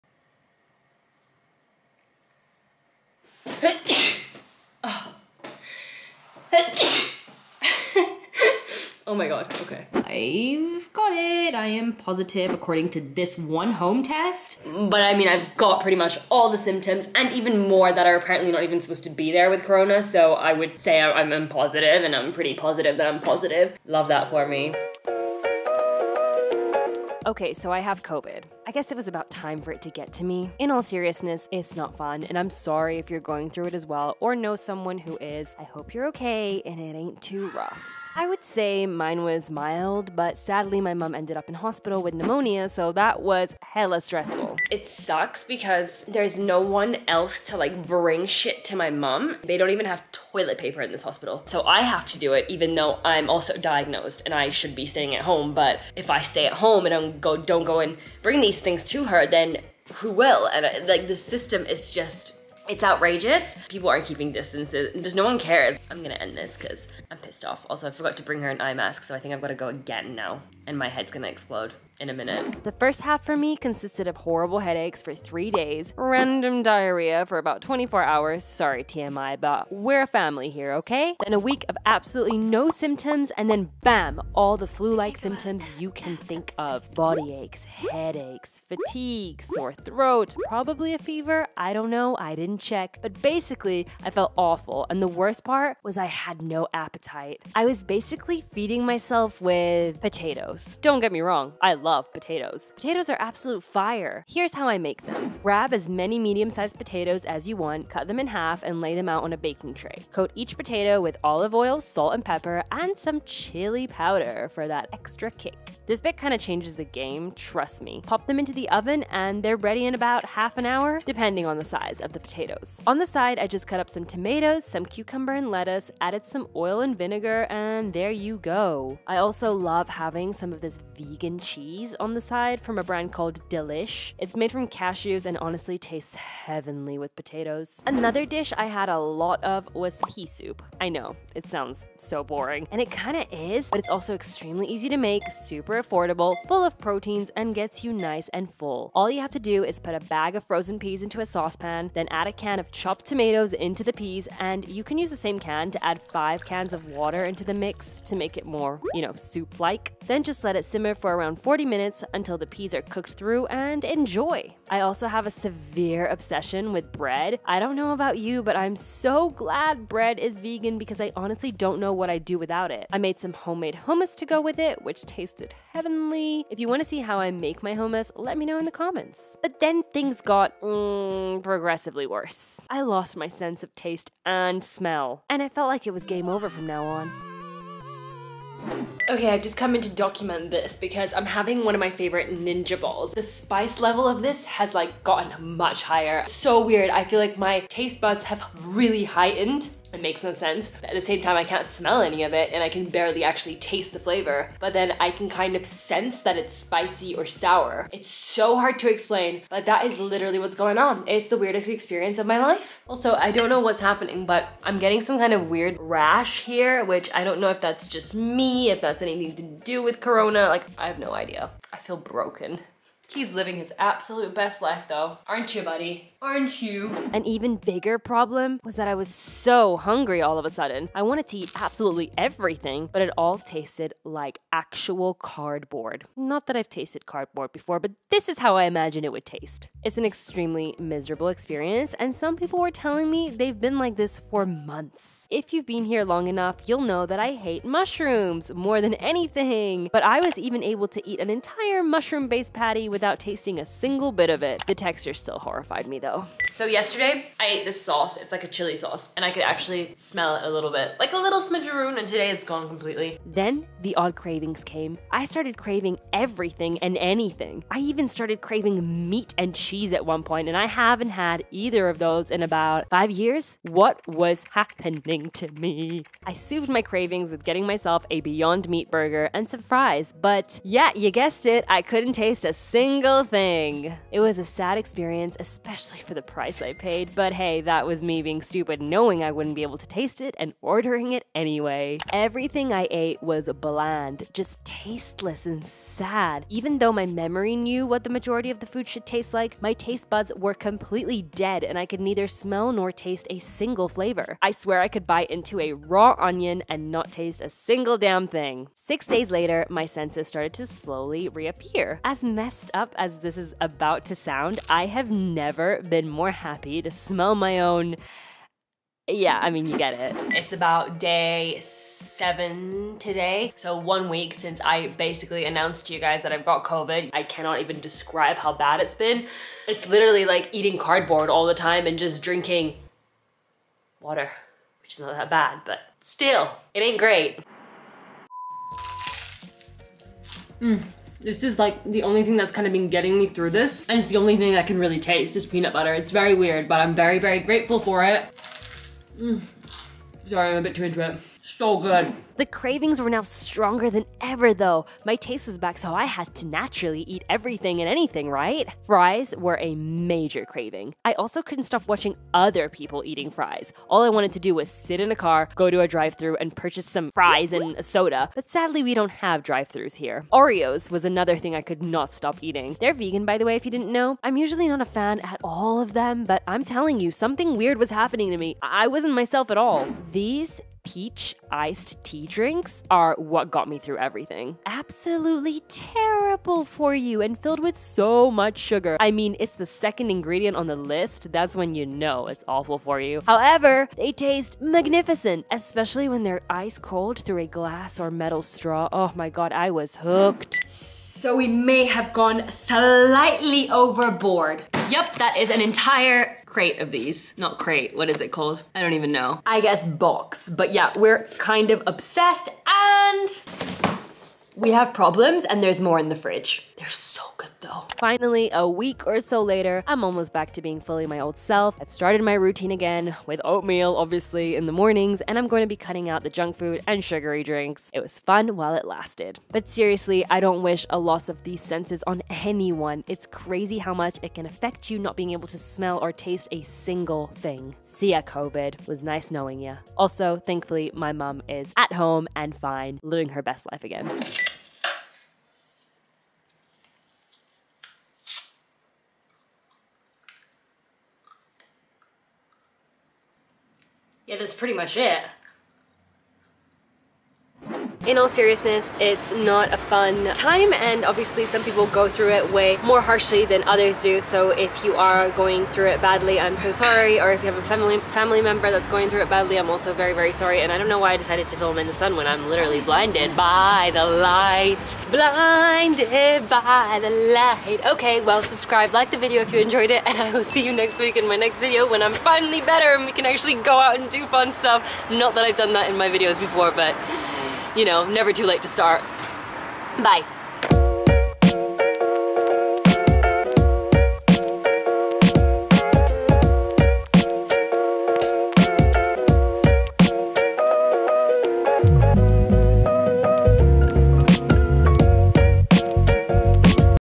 I have COVID-19...Here's What I'm Eating! // Loss of Smell and Taste!!
So, I tested positive for Coronavirus. As many others I lost my sense of taste and smell and it's honestly been an awful experience. Here's me sharing my journey with Covid19 and the process of what I ate throughout.There were some weird cravings!!